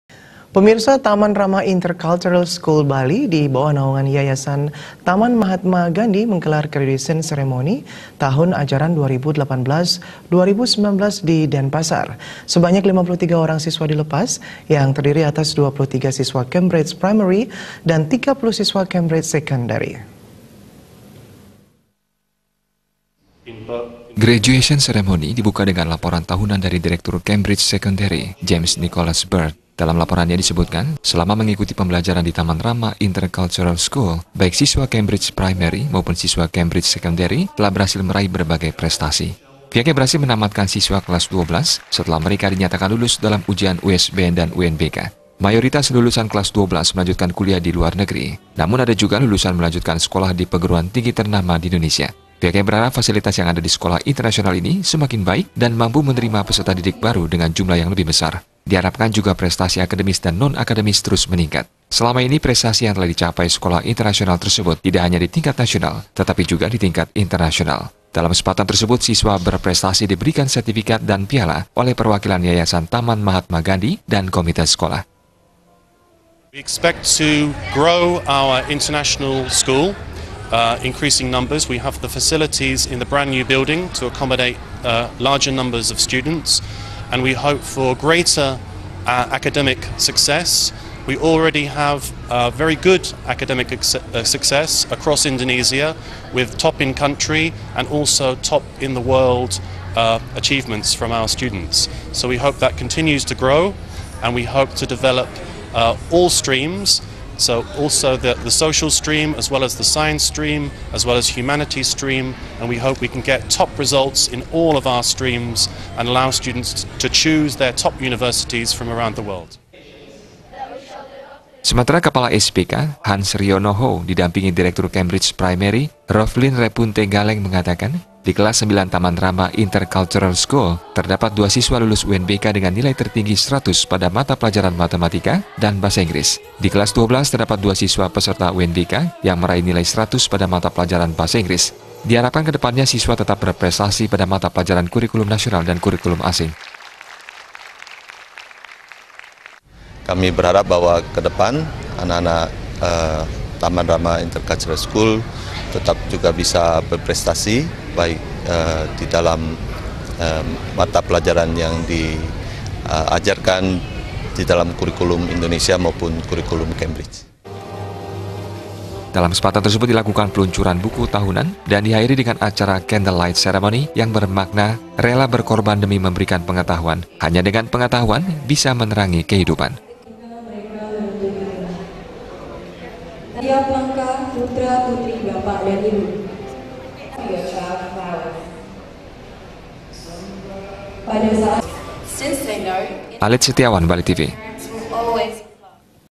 TAMAN RAMA INTERCULTURAL SCHOOL GELAR GRADUATION CEREMONY
TAMAN RAMA INTERCULTURAL SCHOOL BALI, DI BAWAH NAUNGAN YAYASAN TAMAN MAHATMA GANDHI MENGGELAR GRADUATION CEREMONY TAHUN AJARAN 2018,2019, DI DENPASAR. SEBANYAK 53 ORANG SISWA DILEPAS, YANG TERDIRI ATAS 23 SISWA CAMBRIDGE PRIMARY (SD) DAN 30 SISWA CAMBRIDGE SECONDARY (SMA).